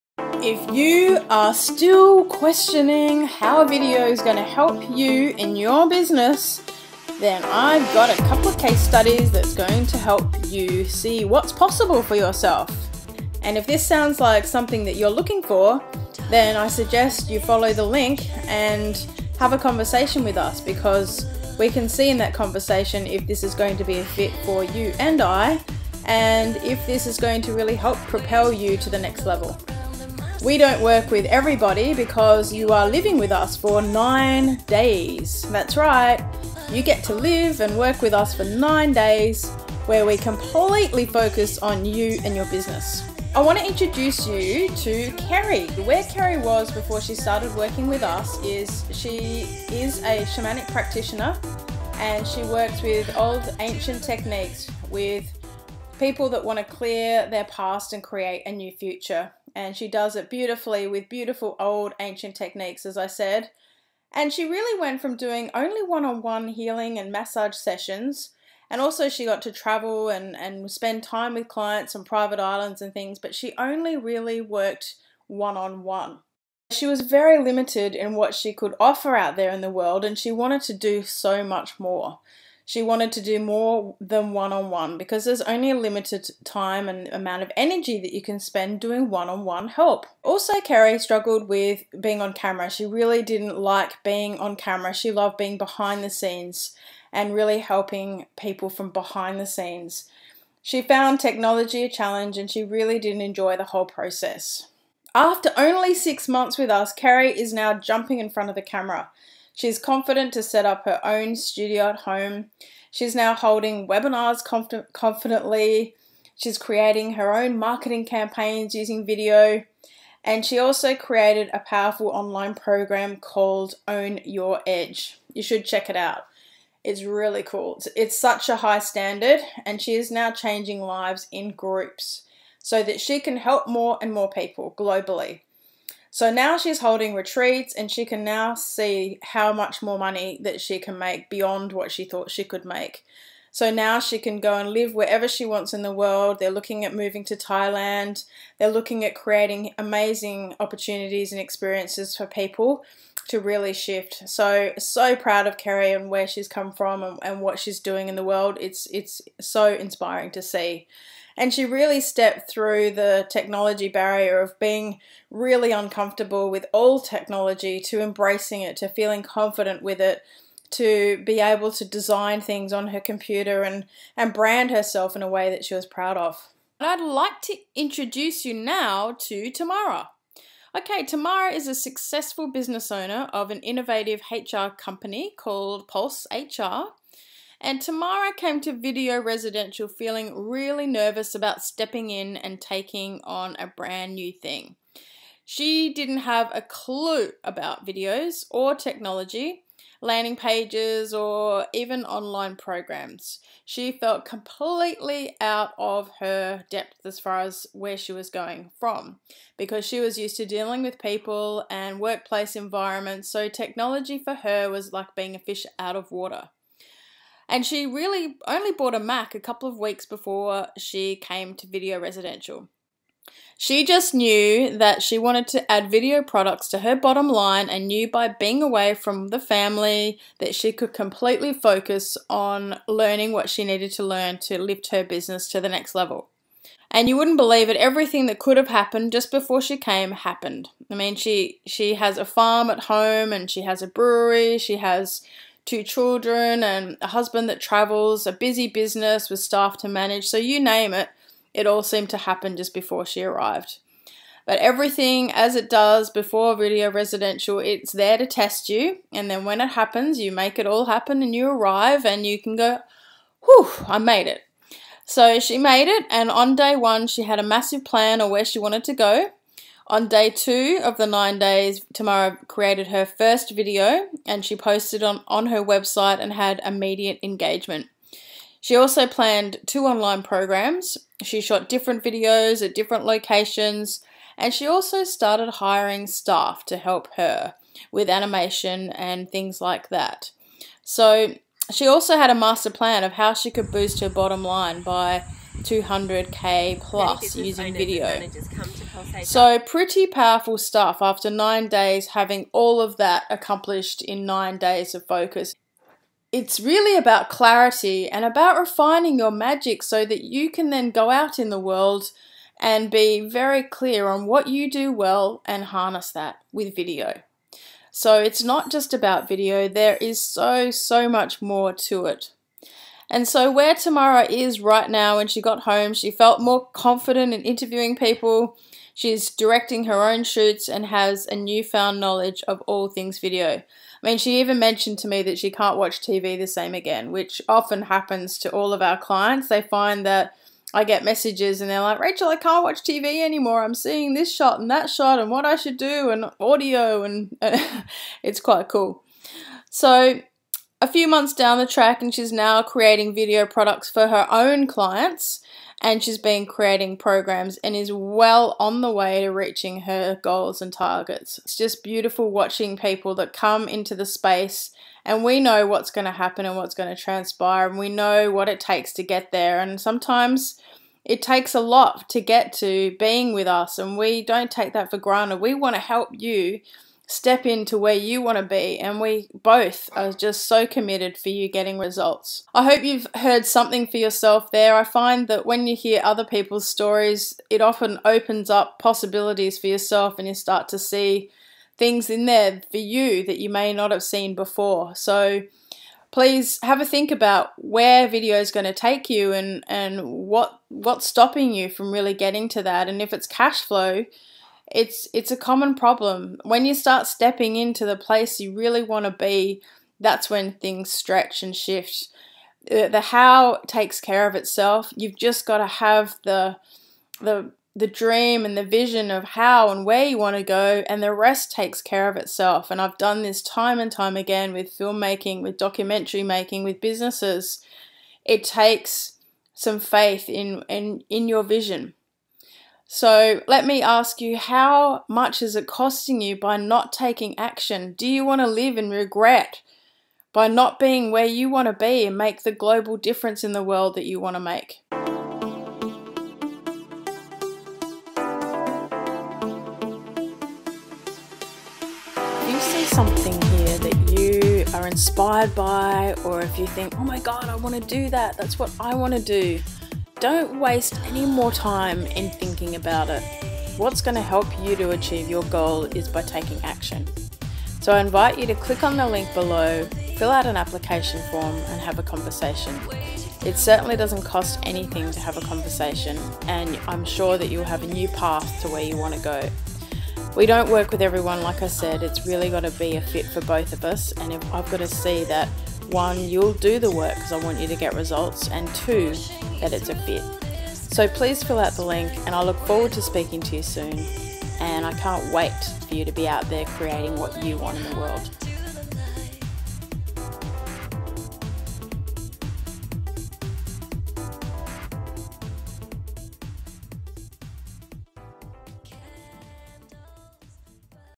Client Case Studies
Want to know how video can work for you? Check out this case studies video where you'll meet Kerri and Tamara. Look at what they have done since working with us and where video has taken them.

When you watch someone else's story, it really opens up possibilities for yourself.